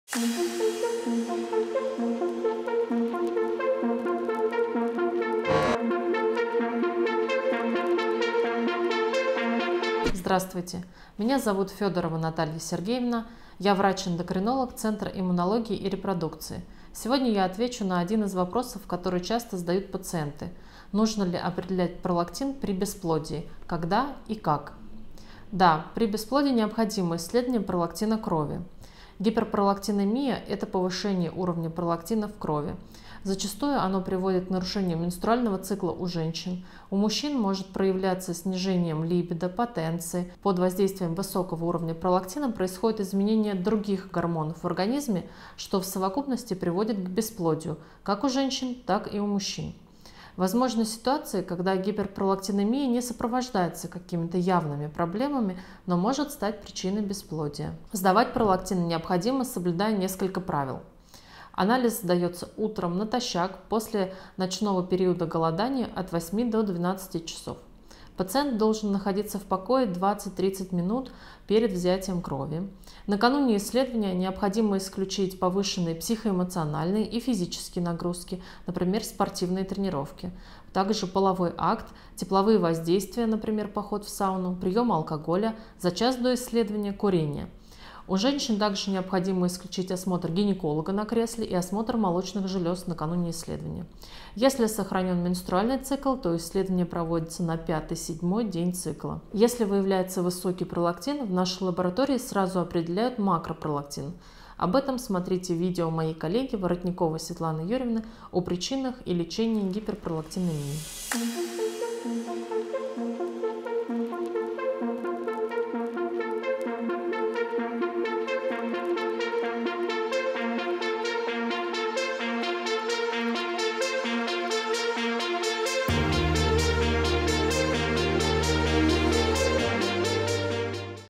АНАЛИЗ НА ПРОЛАКТИН ПРИ БЕСПЛОДИИ. Когда и как сдавать анализ на пролактин.
Анализ на пролактин. В этом видео врач-эндокринолог ЦИР Фёдорова Наталья Сергеевна рассказывает про анализ на пролактин.
При бесплодии необходимо исследование пролактина крови. Гиперпролактинемия — это повышение уровня пролактина в крови, которое зачастую приводит к нарушению менструального цикла у женщин. У мужчин гиперпролактинемия проявляется снижением либидо, потенции, также под воздействием высокого уровня пролактина происходят изменения других гормонов в организме, что в совокупности приводит к бесплодию как у женщин, так и у мужчин. Возможны ситуации, когда гиперпролактинемия не сопровождается какими-то явными проблемами, но может стать причиной бесплодия.
Сдавать пролактин необходимо, соблюдая несколько правил. Анализ сдается утром натощак после ночного периода голодания от 8 до 12 часов. Пациент должен находиться в покое 20-30 минут перед взятием крови.
Накануне исследования необходимо исключить повышенные психоэмоциональные и физические нагрузки (спортивные тренировки), половой акт, тепловые воздействия (сауну), приём алкоголя, за час до исследования – курение. У женщин также необходимо исключить осмотр гинеколога на кресле и осмотр молочных желез. Если сохранен менструальный цикл, то исследование проводится на 5-7 день цикла.
Если выявляется высокий пролактин, в нашей лаборатории сразу определят макропролактин, об этом смотрите видео моей коллеги Воротниковой Светланы Юрьевны о причинах и лечении гиперпролактинемии.

Записаться на консультацию вы можете, обратившись в наш Центр по многоканальному телефону или на нашем сайте, пройдя по ссылке
При первичном посещении не забудьте предъявить купон на скидку, который находится